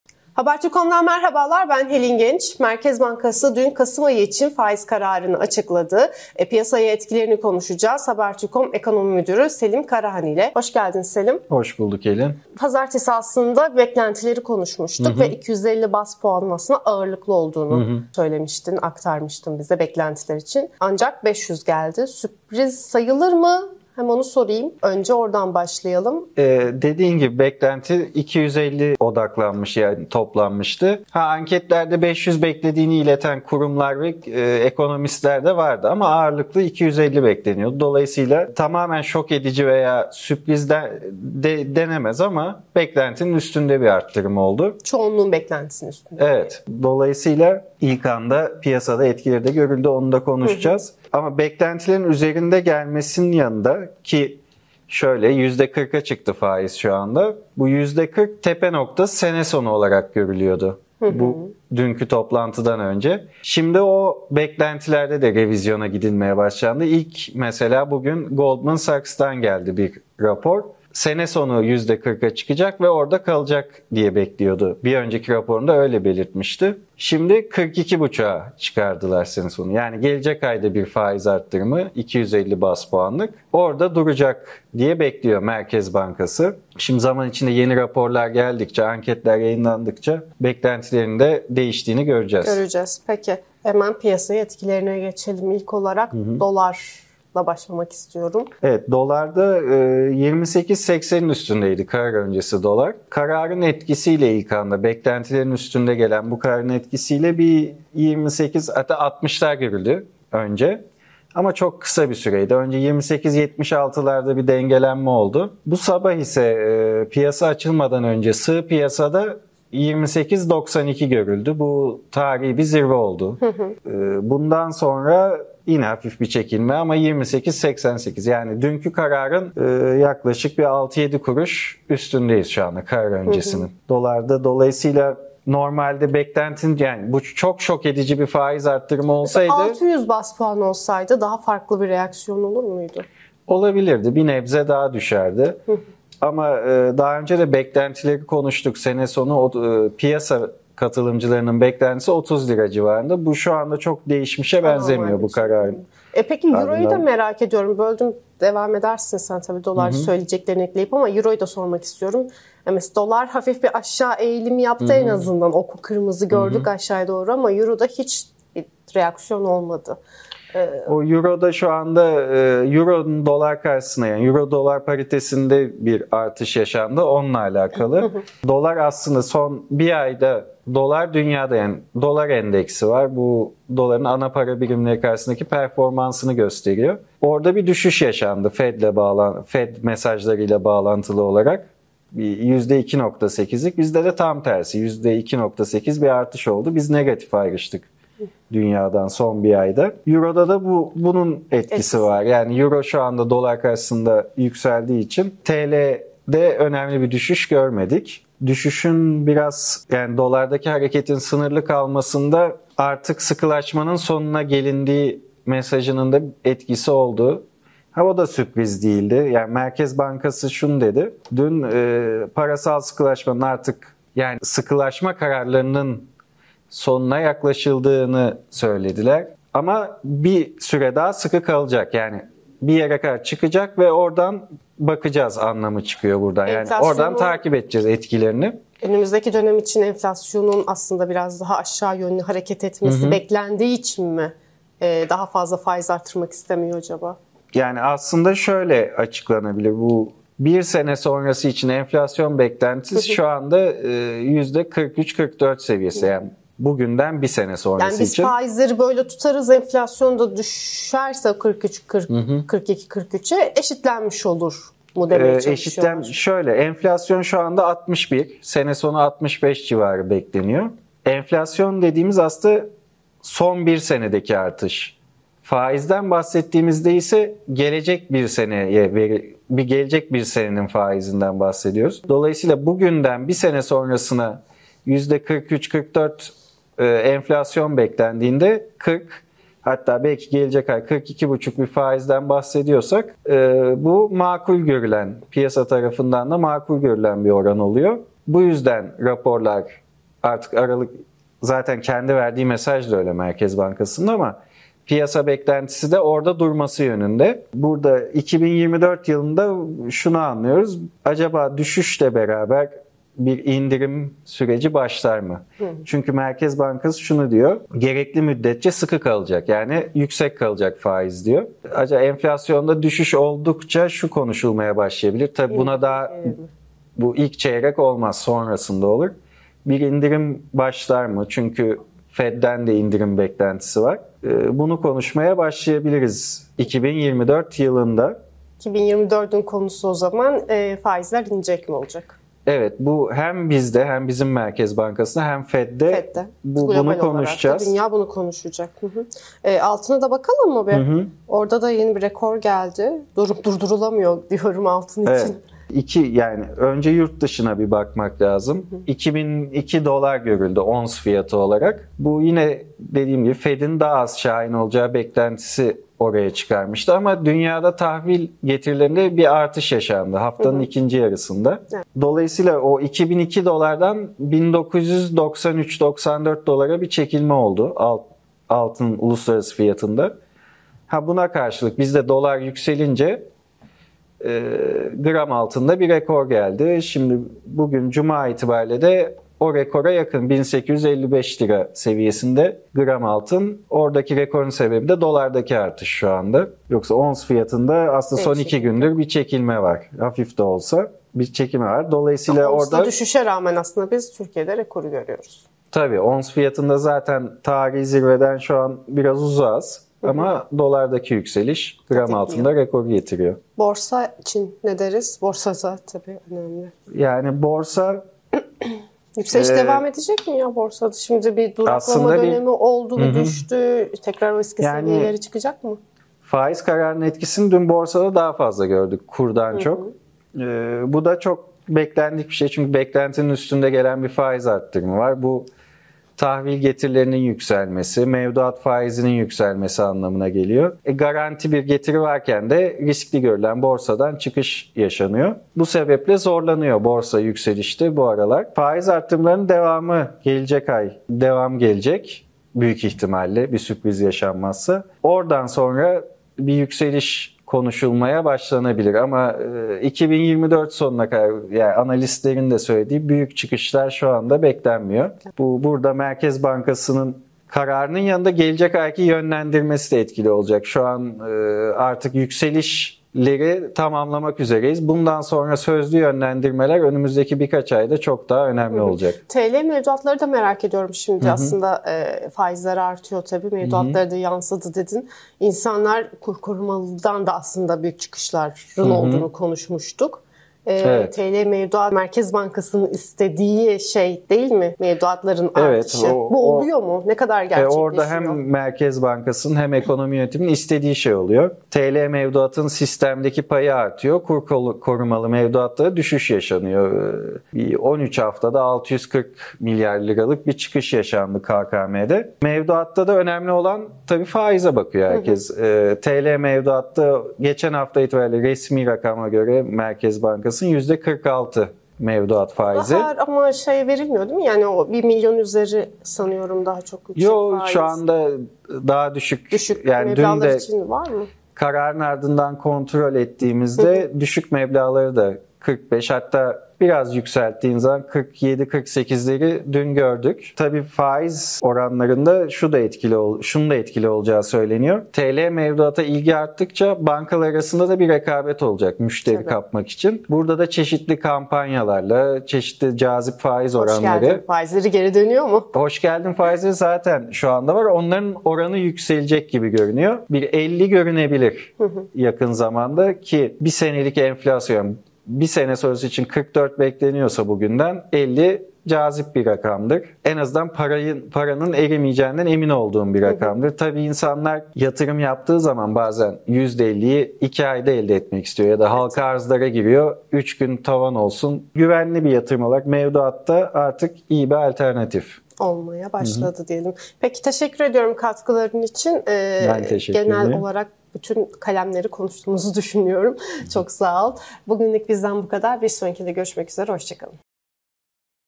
Merkez Bankası'nın faiz kararına kur ve altın nasıl tepki verdi? Vadeli mevduat faizleri kaç oldu?
Merkez Bankası (TCMB) Kasım ayında 500 baz puanlık faiz artışına gitti. Peki Merkez Bankası'nın faiz kararı dolar, euro ve altında nasıl etkili oldu? Borsa, faiz kararı sonrası düştü mü, yükseldi mi? Bankalarda TL vadeli mevduat hesabı faizleri kaç oldu? HT Mikrofon'da Helin Genç sordu, Haberturk.com Ekonomi Müdürü Selim Karahan yanıtladı...